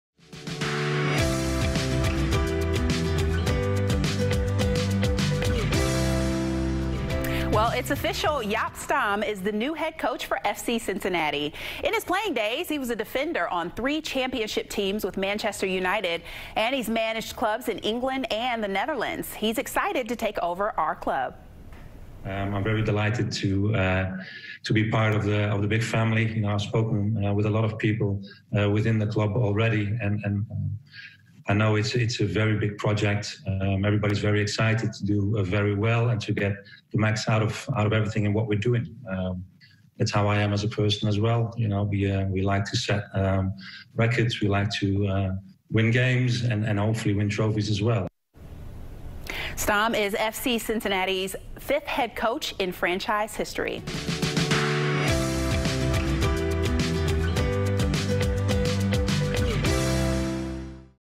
May 23, 2020: FC Cincinnati hires its next head coach
Segment from First Financial Bank Biz Beat presented by the Cincinnati Business Courier.